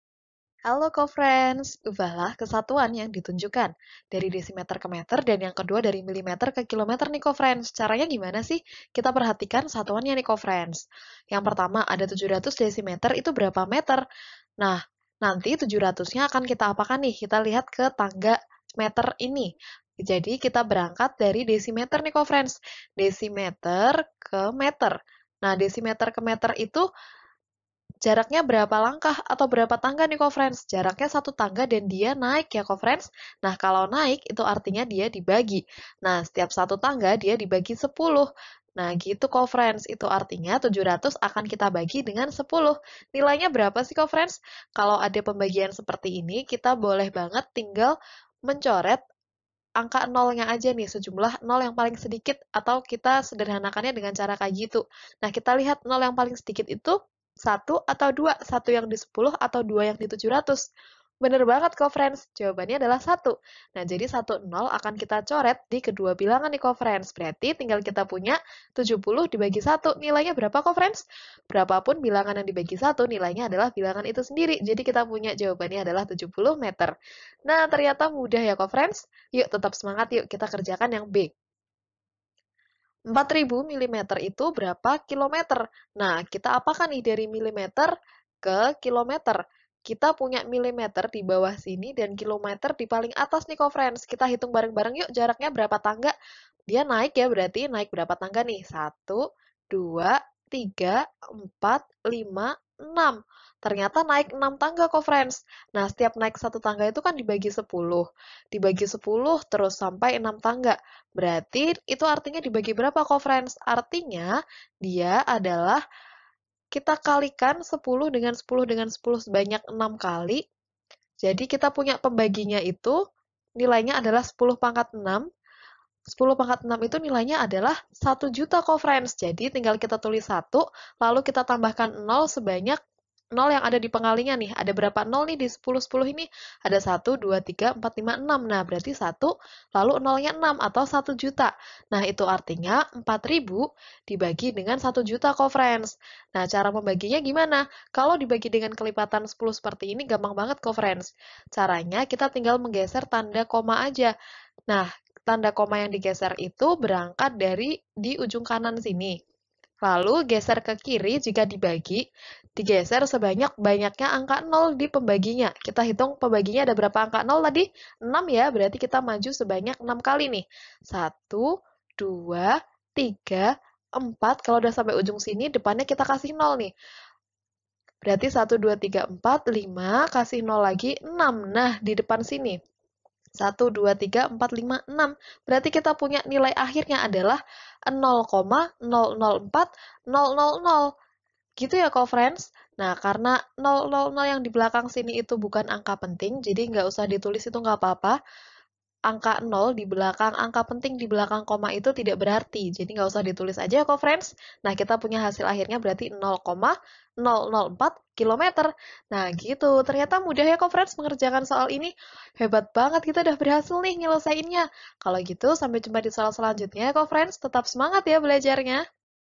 Ubahlah ke satuan yang ditunjukkan. a. 700 dm = ... m b. 4.000 mm = ... km
Ubahlah ke satuan yang ditunjukkan.
 a. 700 dm = ... m
 b. 4.000 mm = ... km

Topik atau Materi: Satuan Panjang - Satuan Baku - Aritmatika - Matematika Kelas 3 SD - K-13 Revisi

Sumber Soal: Matematika Kelas 4, K-13 Revisi, MK, 2019 , Satu Nusa

00:00 : Bedah Soal
01:00 : Konsep, rumus dan pengertian Satuan Panjang
01:18 : Langkah penyelesaian soal
02:39 : Jawaban akhir
03:09 : Penutup